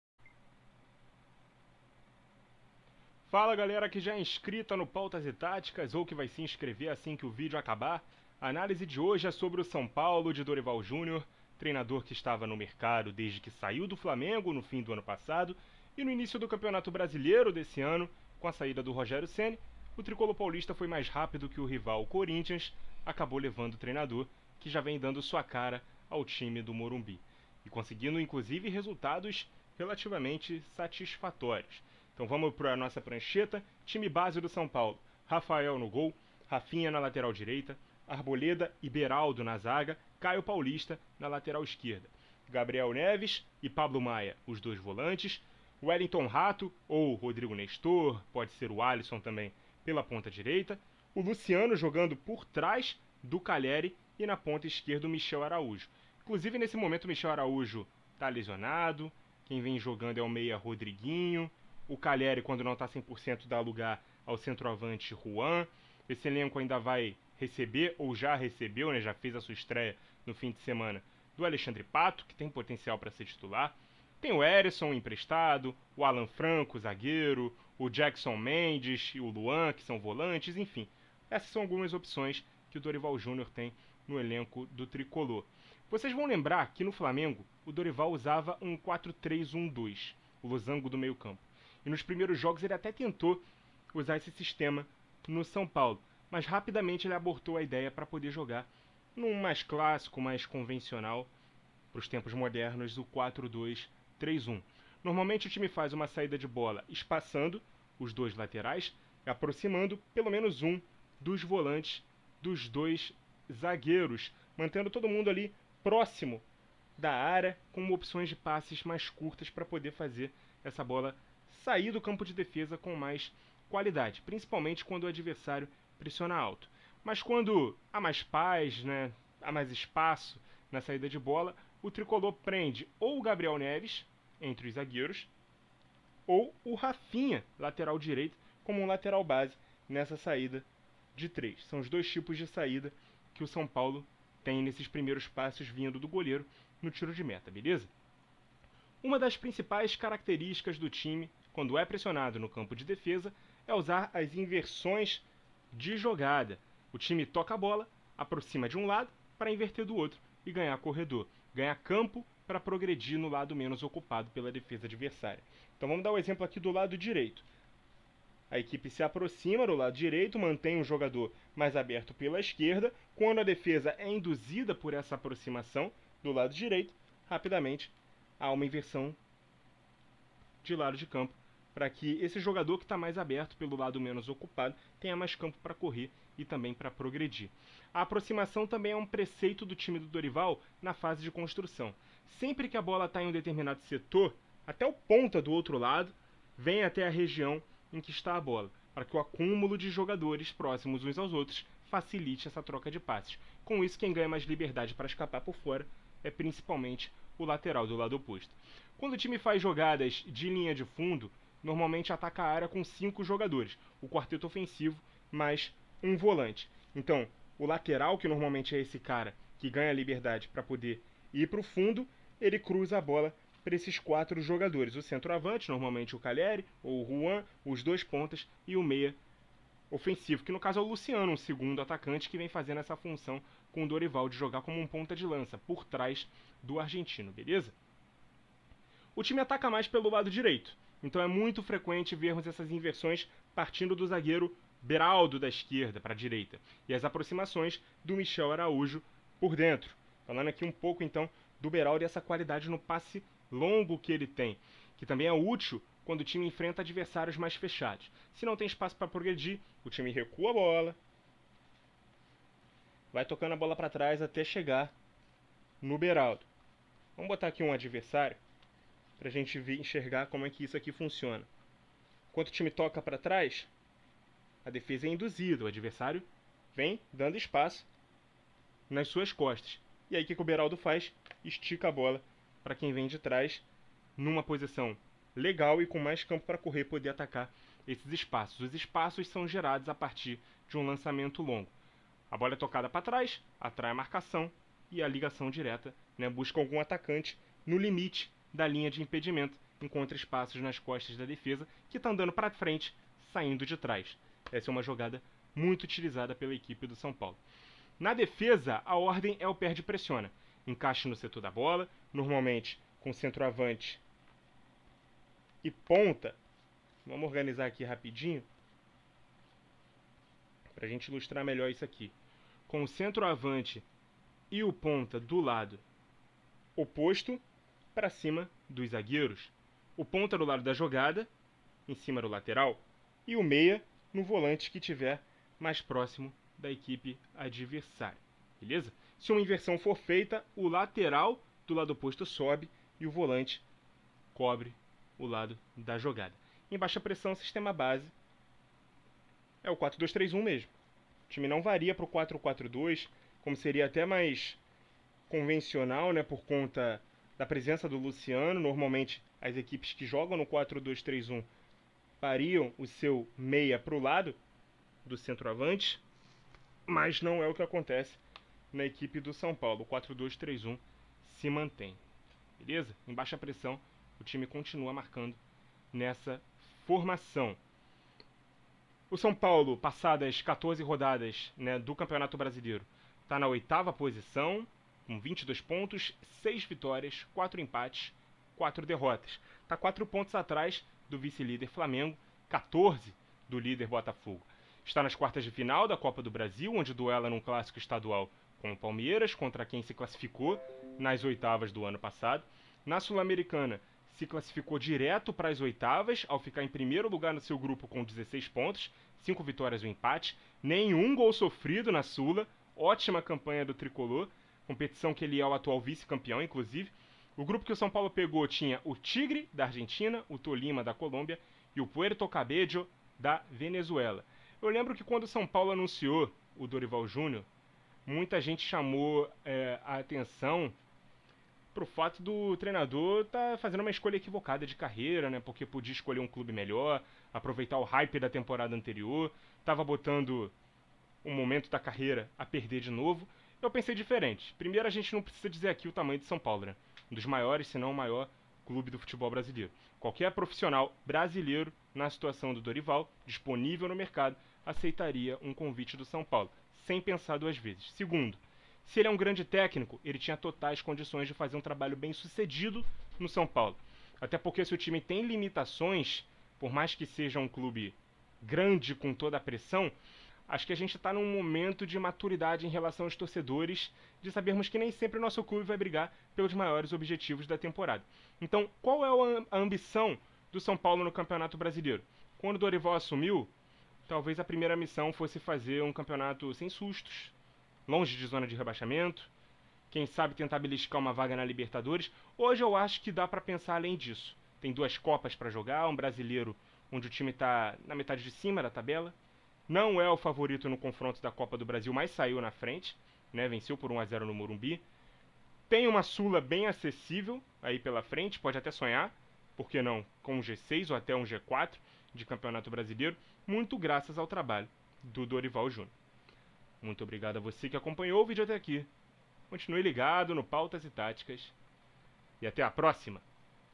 Análise Rápida: São Paulo de Dorival Jr (2023)
DORIVAL JR ASSUME O SÃO PAULO E RAPIDAMENTE IMPRIME SUA IMPRESSÃO DIGITAL NO JEITO DO TIME JOGAR. O TRICOLOR FAZ BRASILEIRO REGULAR E ESTÁ VIVO NAS COPAS DO BRASIL E SUL-AMERICANA.